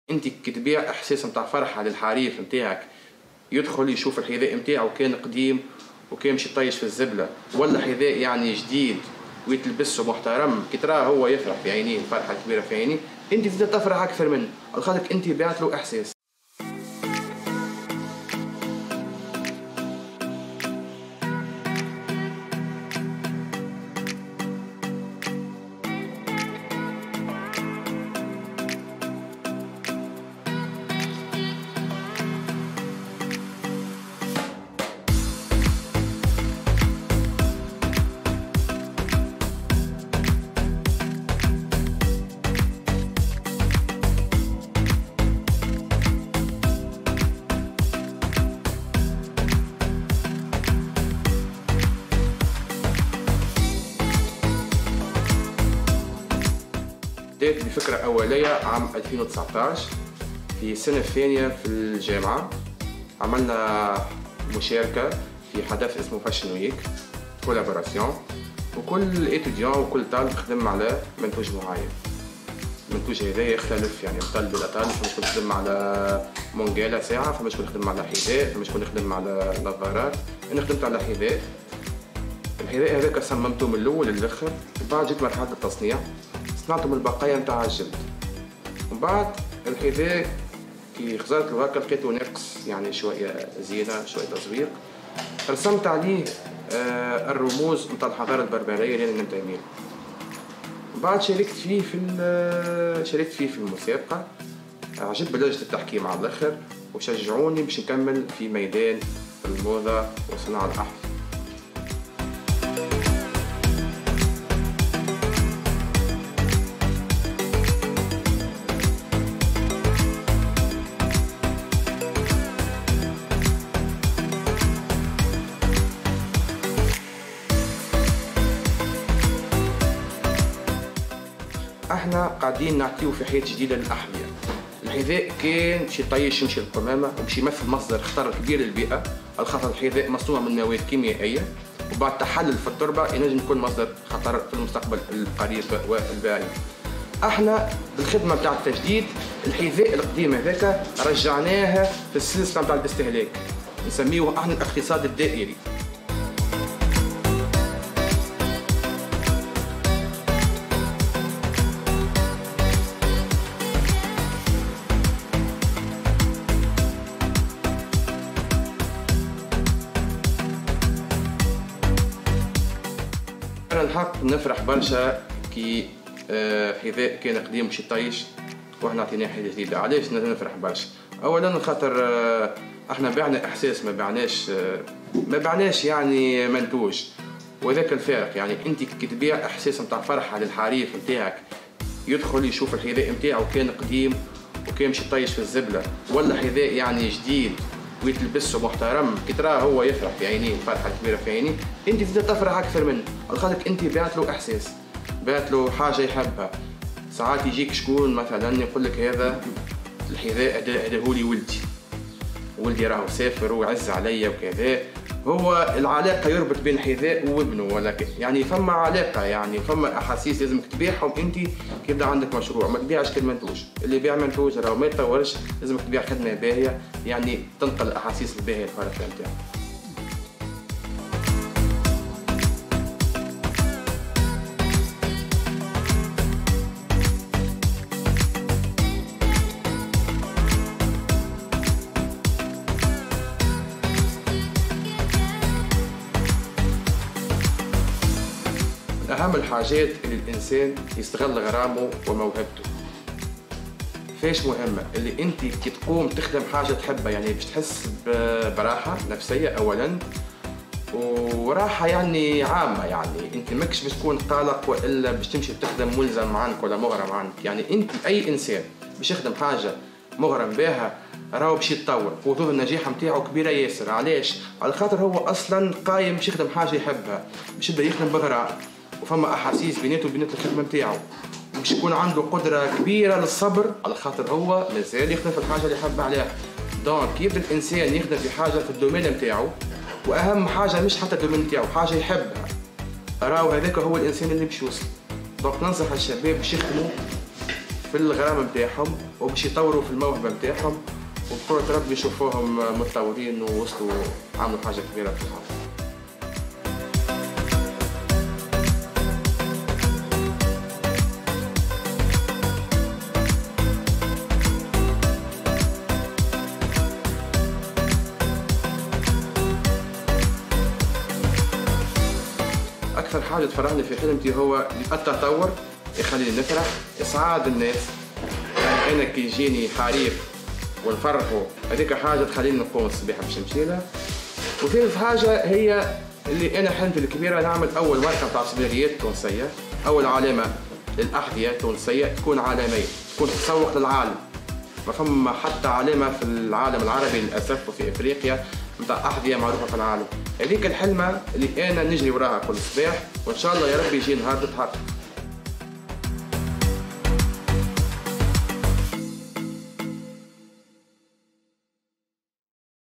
شاب تونسي يبدع في تحويل الأحذية المستعملة إلى تحف فنية
شريف زروي ... شاب تونسي يبدع في تحويل الأحذية المستعملة إلى تحف فنية تنبض بالحياة... تعرف على قصته في هذا الفيديو